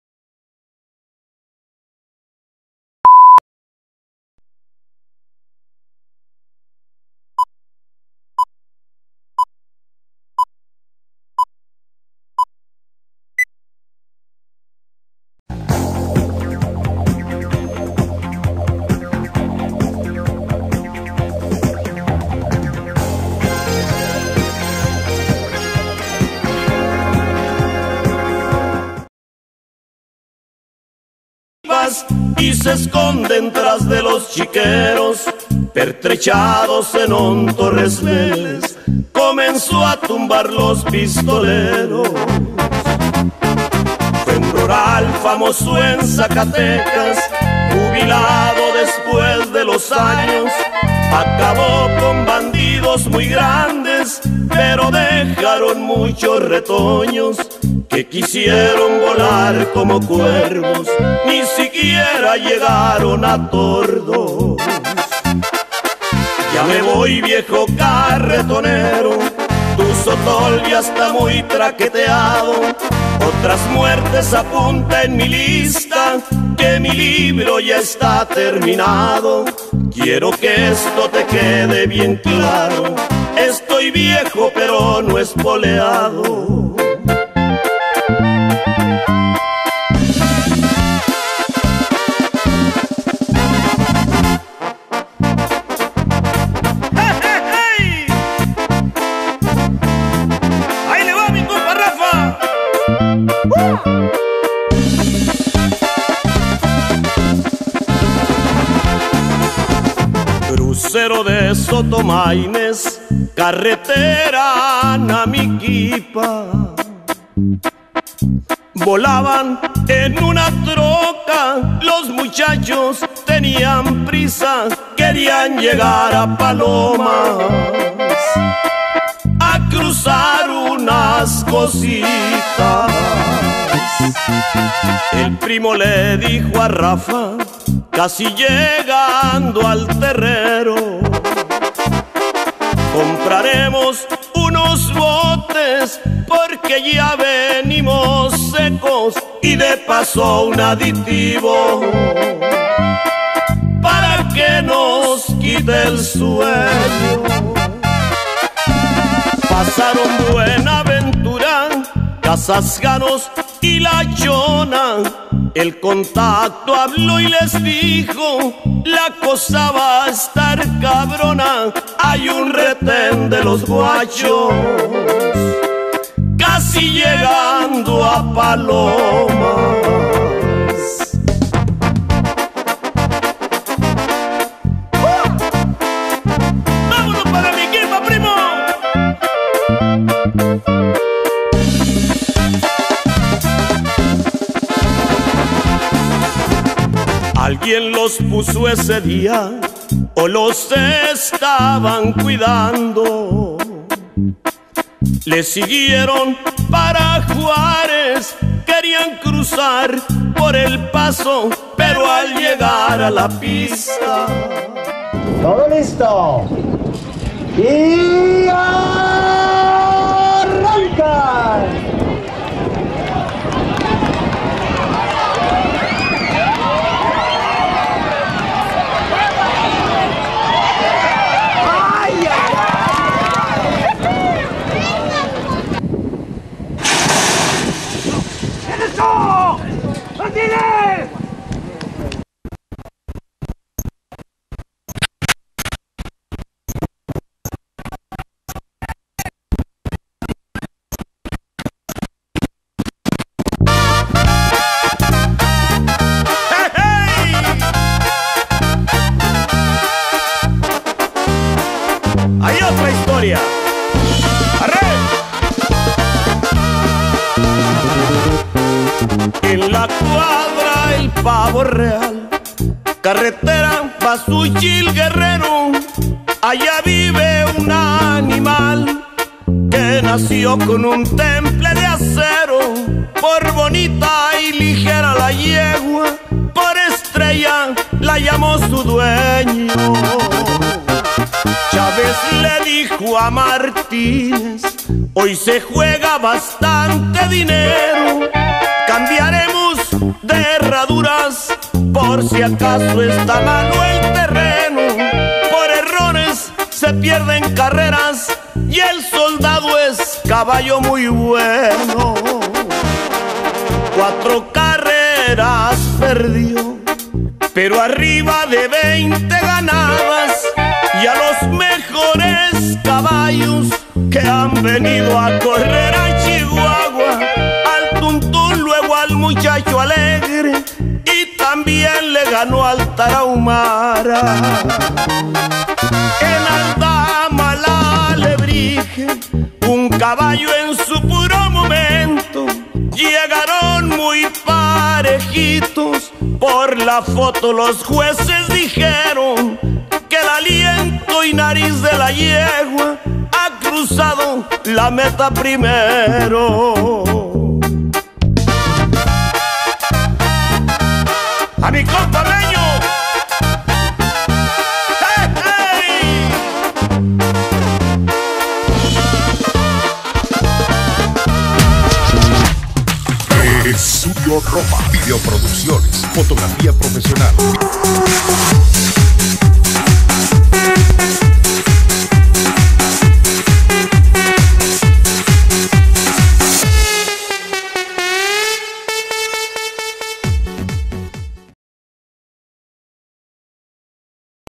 La Pantera Vs El Oscarito
Carreras de Caballos en el Carril La Guajolota el dia 8 de Abril del 2012